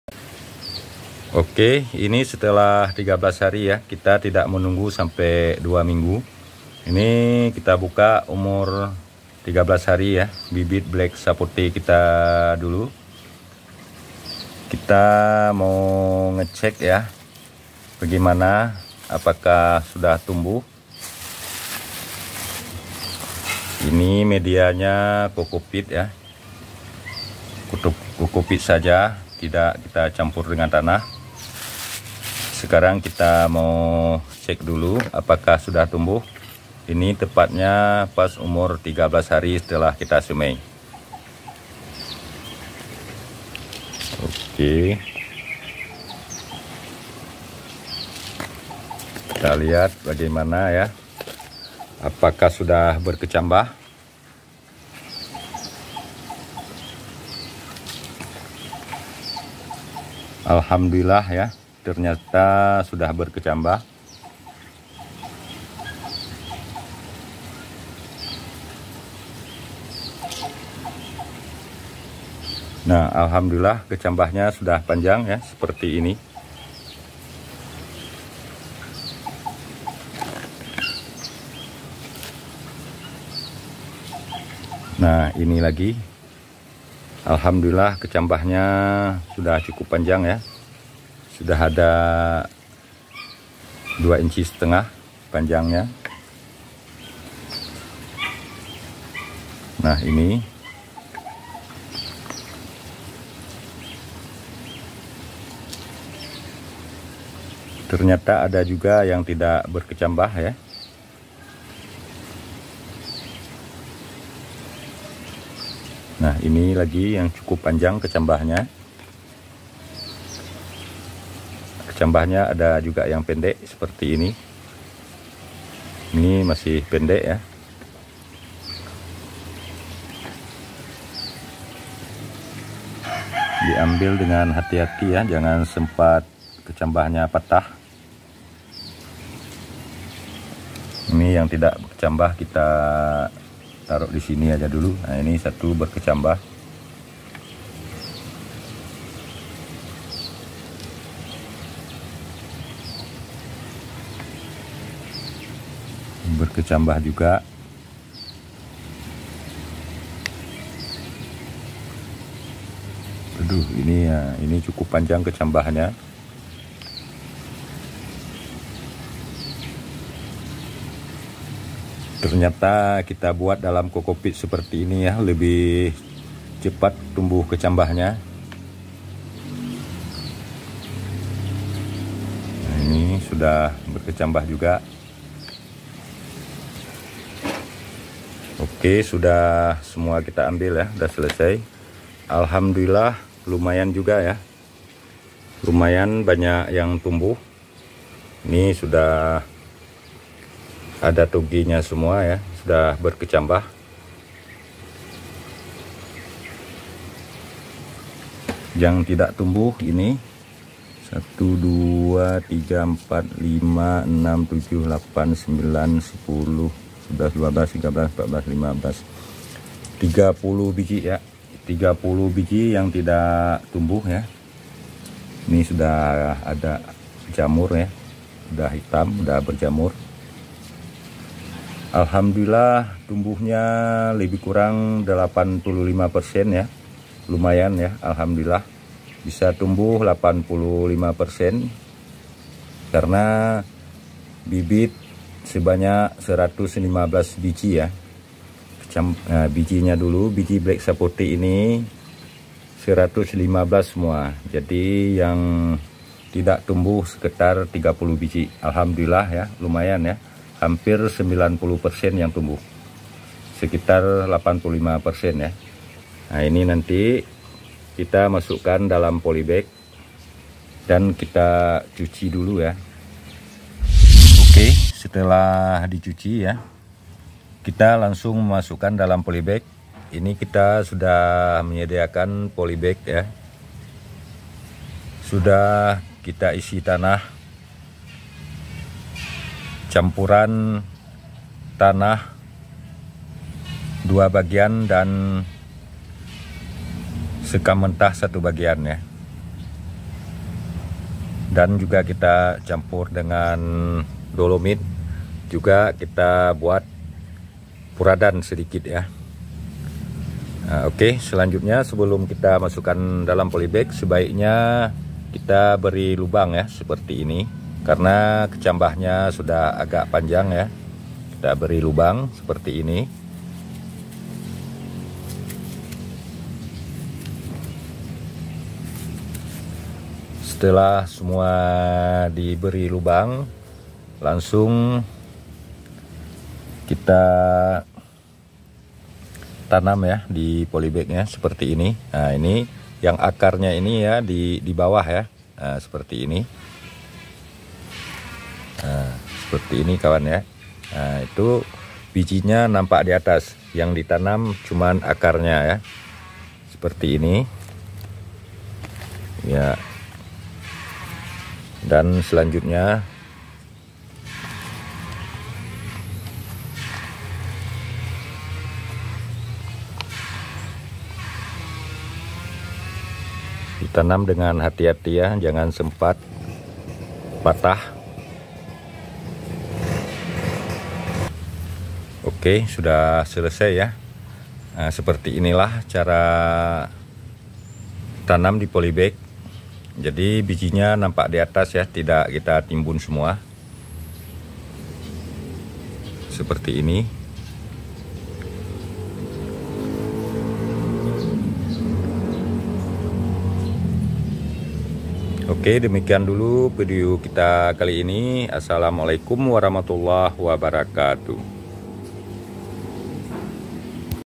melihat hasil penyemaian sendiri biji black sapote beberapa minggu lalu
cara ini adalah cara paling efektif menurut kami, dikarenakan sulitnya tingkat keberhasilan dengan cara pencangkokan, sehingga kami melakukan penyemaian biji black sapote lokal sebagai batang induk yang akan kami sambung dengan batang tua yang telah beberapa kali berbuah (blacksapote jumbo) yang ada di halaman rumah kami.